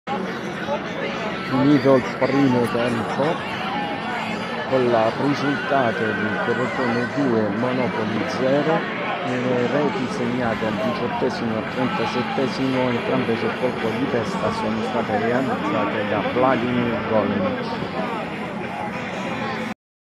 M91CROTONE LIVE: IL TACCUINO FLASH DI ANTONIO MILANO - CROTONE MONOPOLI 2° TURNO SERIE C GIRONE C
Flash Crotone Monopoli